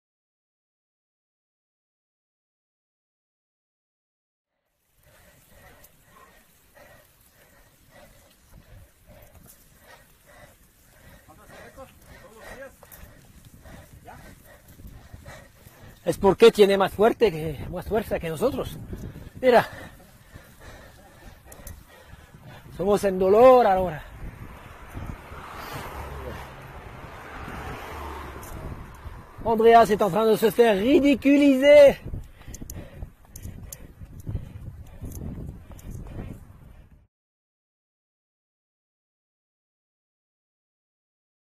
The Colombian Farmer Who Humiliated An Ironman Cyclist
Colombian Farmer, Humiliated, Ironman Cyclist, Bike